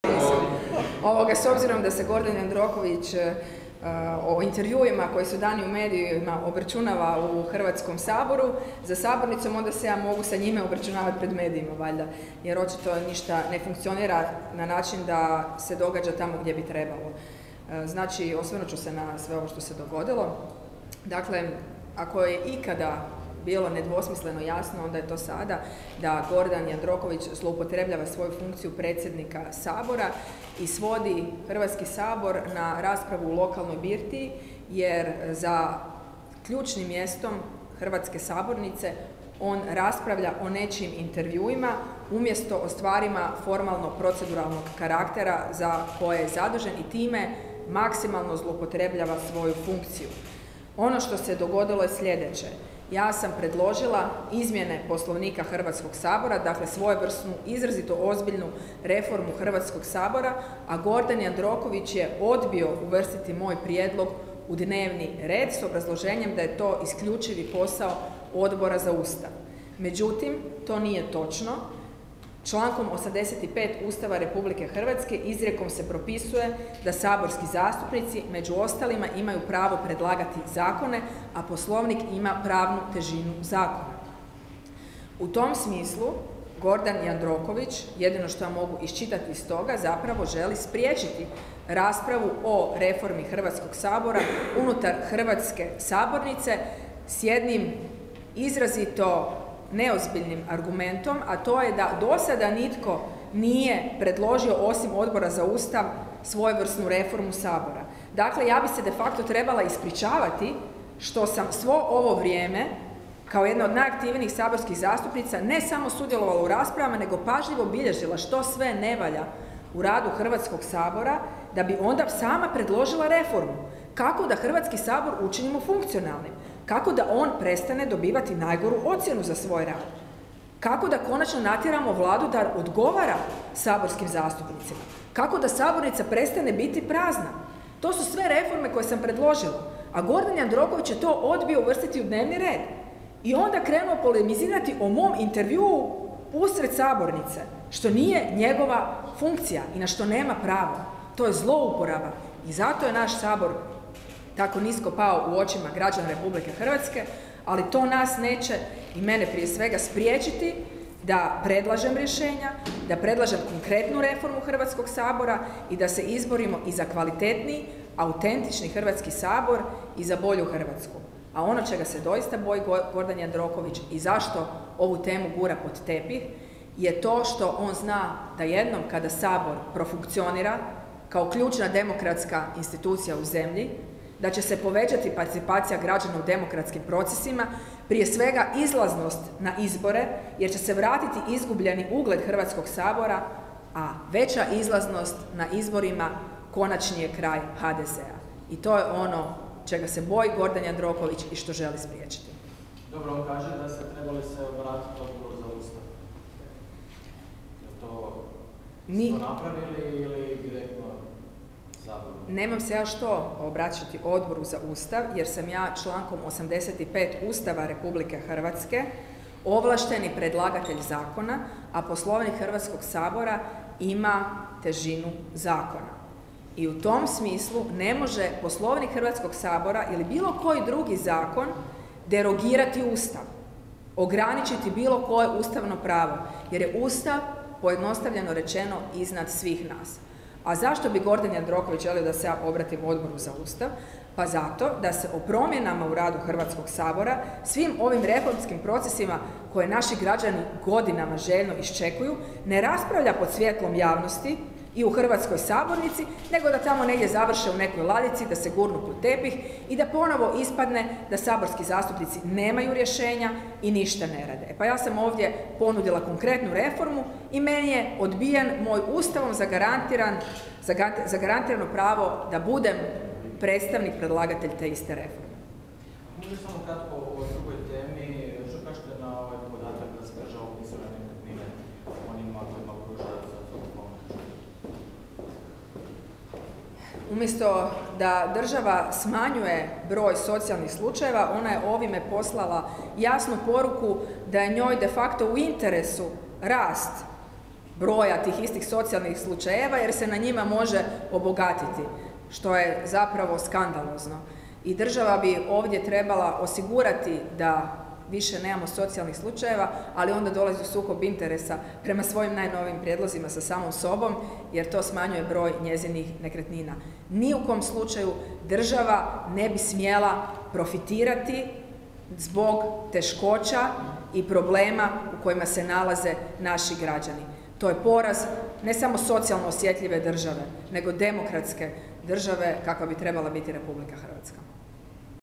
Selak Raspudić: "Jandroković zloupotrebljava svoju funkciju predsjednika Sabora"
Na konferenciji za medije održanoj u Saboru zastupnici Marija Selak Raspudić i Nikola Grmoja komentirali su aktualne teme.